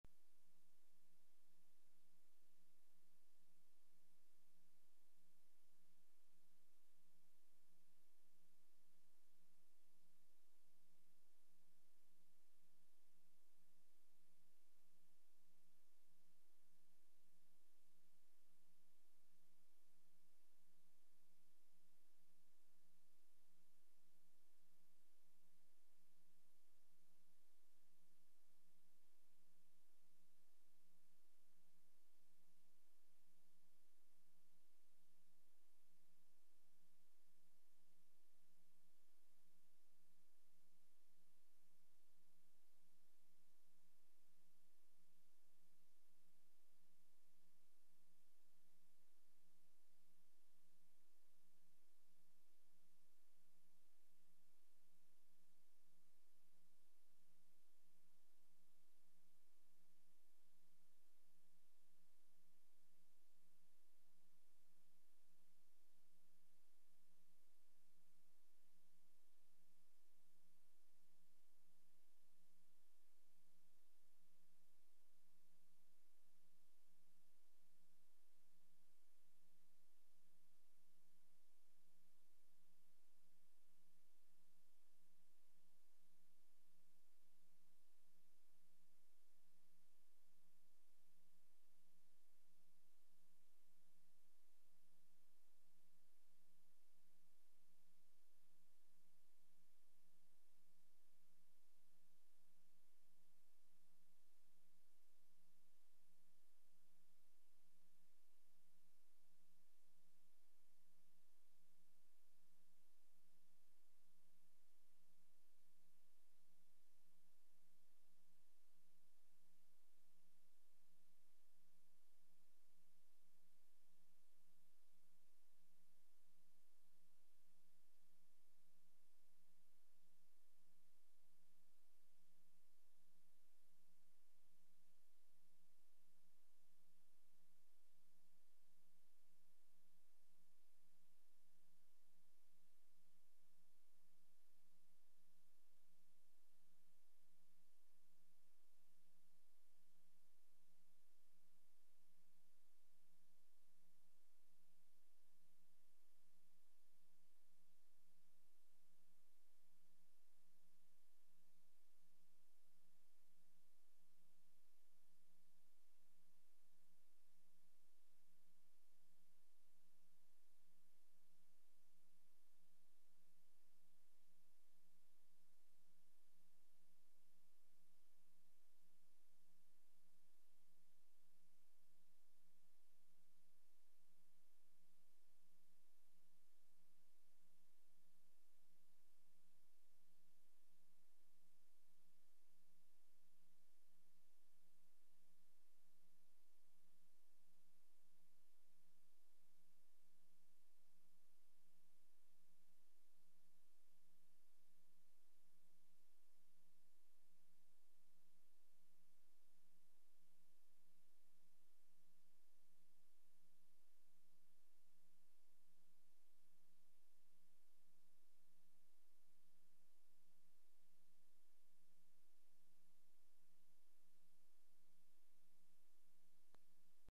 FLYING ARTILLERY (ROCKET TYPHOONS) - NO SOUND
Aerial shots of Typhoons firing 2, 4 & 8 rockets simultaneously. Various shots of rockets leaving planes, also some hitting their targets.